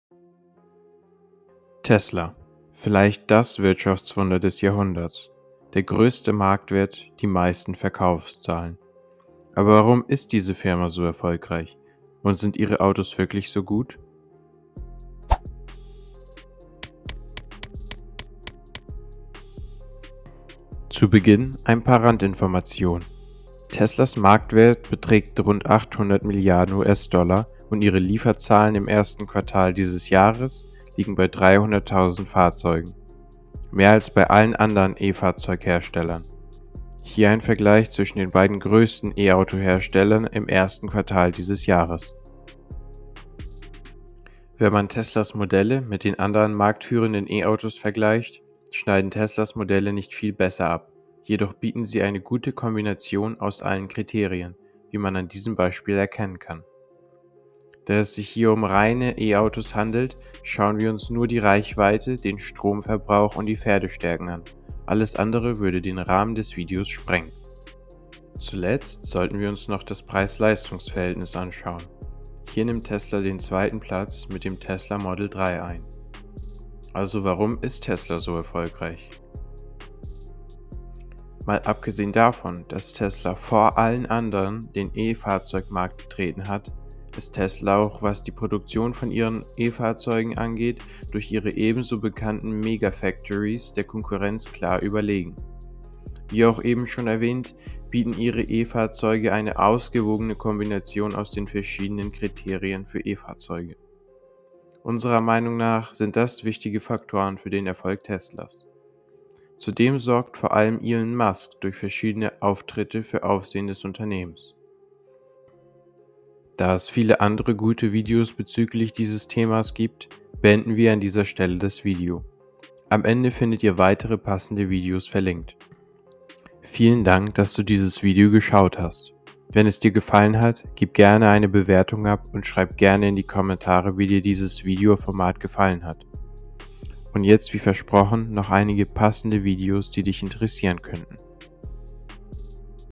Warum Tesla die E-Auto Industrie dominiert?
Ein kurzes und informatives Video über das Unternehmen Tesla und seine Stellung im E-Fahrzeug Markt. Dabei wird ein riesiges Themengebiet in diesem Video eher eingeführt und dient somit auch als Unterhaltung.
Wir hoffen dir gefällt das Video! Schreib gerne ein Kommentar und gibt eine Bewertung ab.
Über ein Abo würden wir uns ebenfalls sehr freuen!